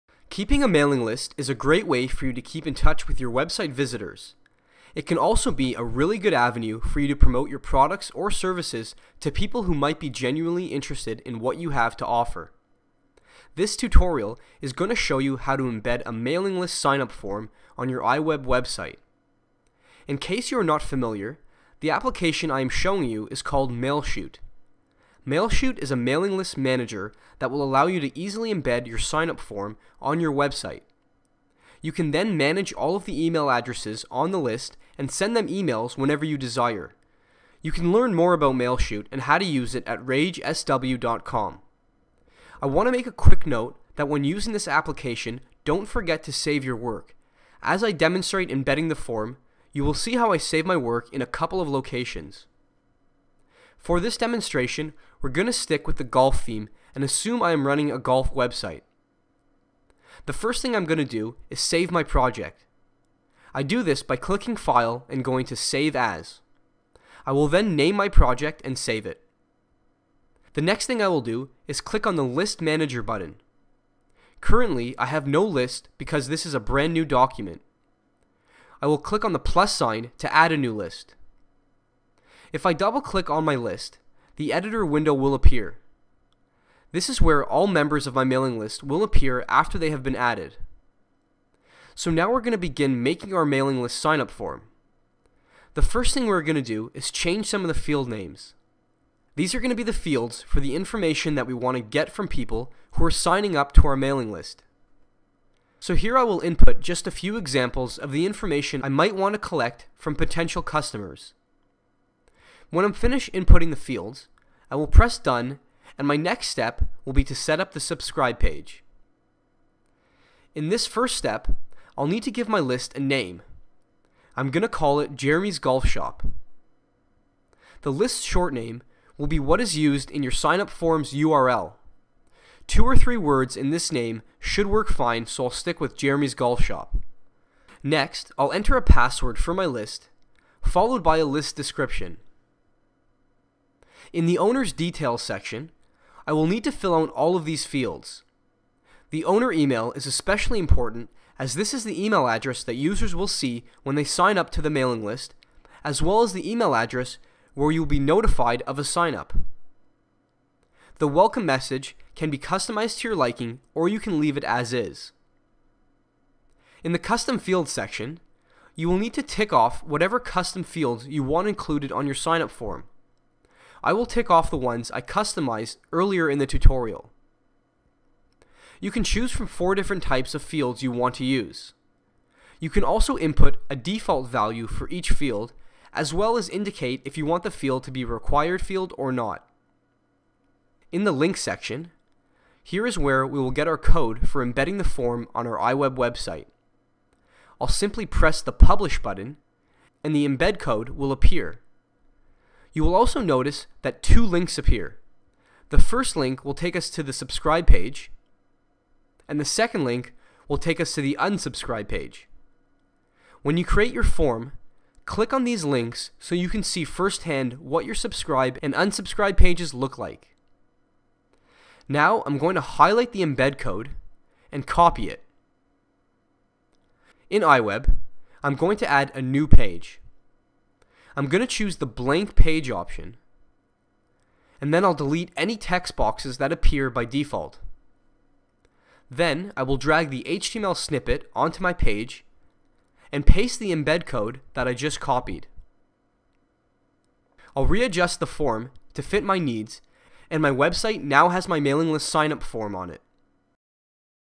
How To Put A Newsletter Signup Form On iWeb Using MailShoot
Learn how to embed a newsletter sign up form on your iWeb website very easily by using RAGE Software's MailShoot.

MailShoot will let you easily create a form on your website where users can sign up to your mailing list. Everything is handled for you and you don't need to worry about any technical requirements on your end.

Once the form is created, you can import your subscribers into MailShoot and send them personalized emails through the software.